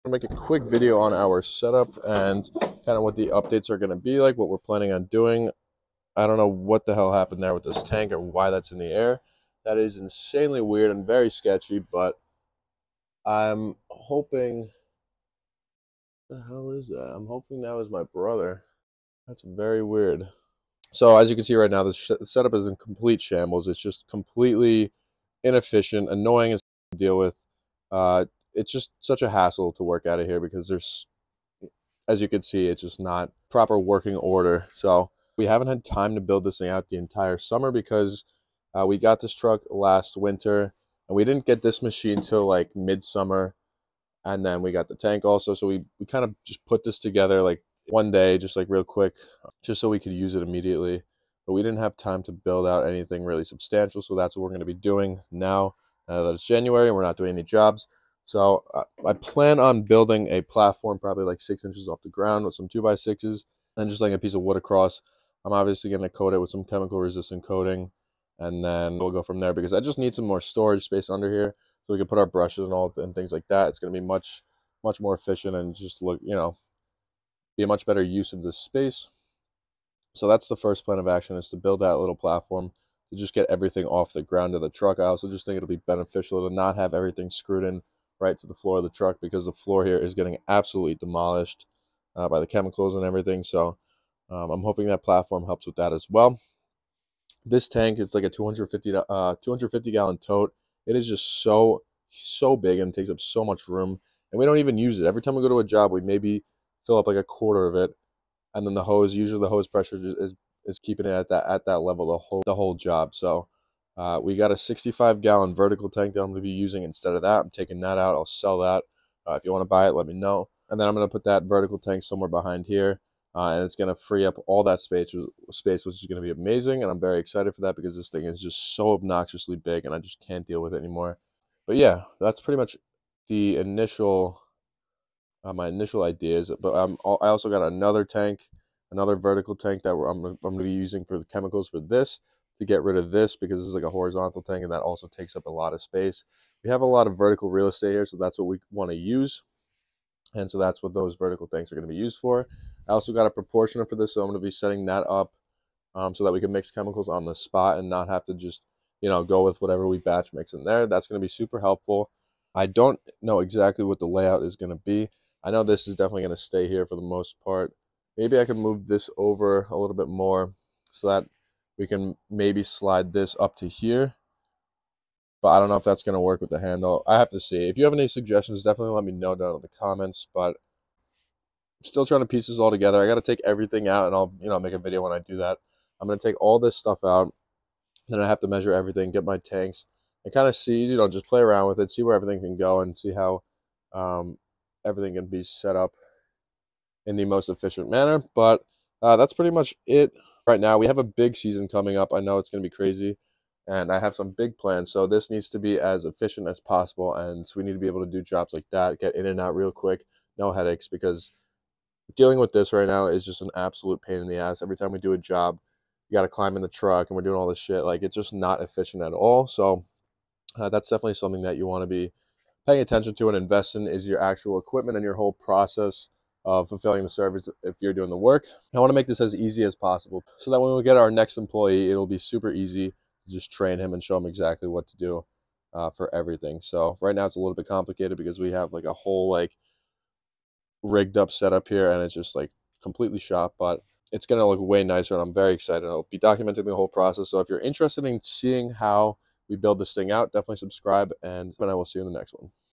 Pressure Washing Truck Setup - Plan for 2023!
This is the current state of our pressure washing setup and I know it’s very ugly but that will all change soon. Our van setup is going to look much better once we’re done revamping it and I will be showing the whole process so if your interested in following along, subscribe, and we’ll be getting after the 2023 pressure washing season.